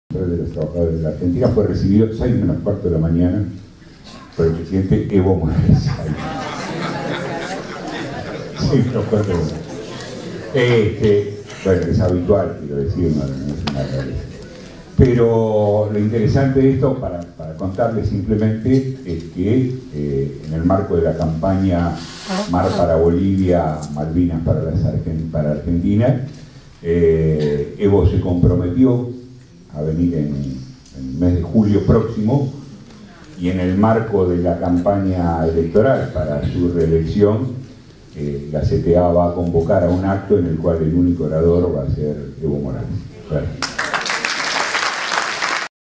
10CA Mar para Bolivia/Malvinas para Argentina
Carlos Girotti.Asamblea de Carta Abierta, sábado 22 de marzo de 2014. Biblioteca Nacional, Buenos Aires.